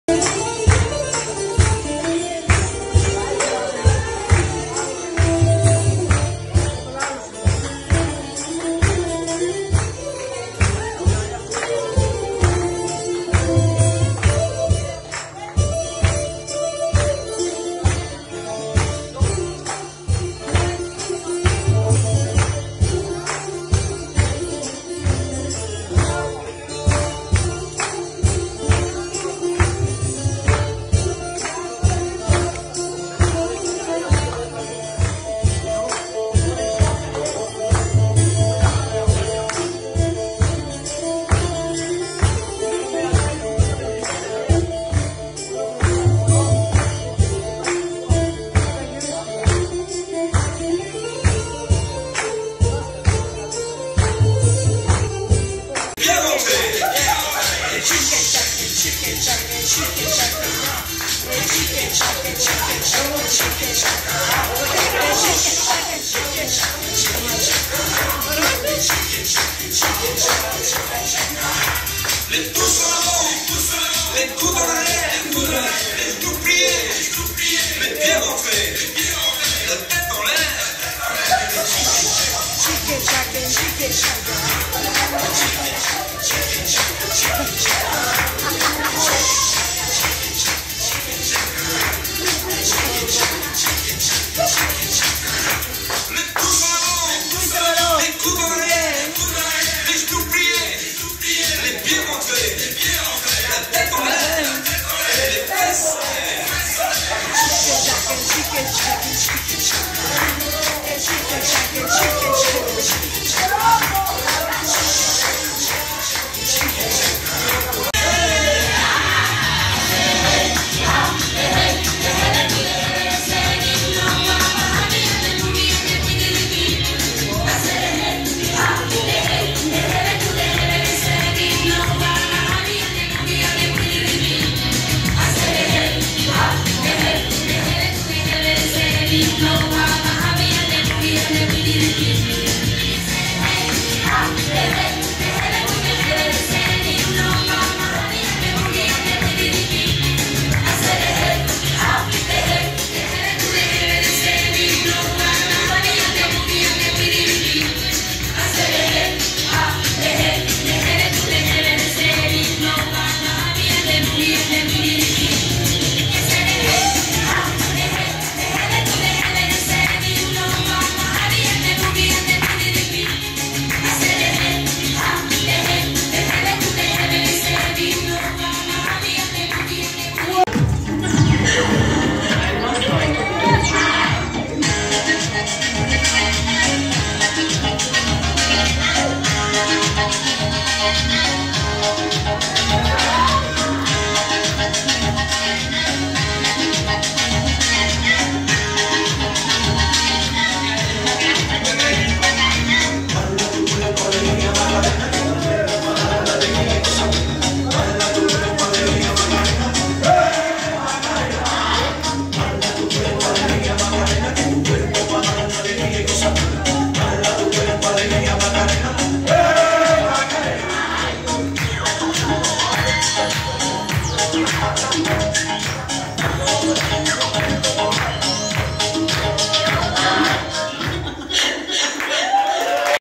Digital competences & reading skills - International evening - Greece 2023
The international evening full of dance, singing and fun 🙂